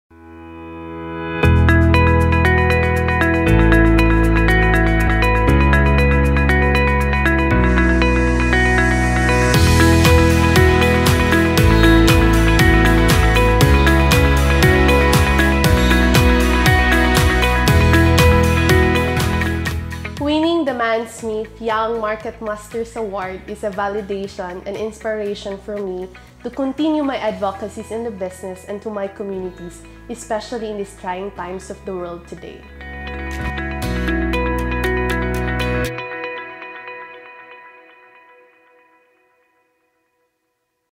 Dalareich Polot (Ginto Fine Chocolates Corporation) 15th Mansmith YMMA 2020 for Advocacy Marketing
Dalareich Polot (Ginto Fine Chocolates Corporation) is one of the 15th Mansmith Young Market Masters Awardees (Mansmith YMMA) for 2020. to know more details.

The Mansmith YMMA is an advocacy project organized and presented by Mansmith and Fielders that seeks and recognizes outstanding marketers and entrepreneurs who are 35 years old and younger and who can serve as the Philippines' role models for outstanding and innovative brand building and entrepreneurship.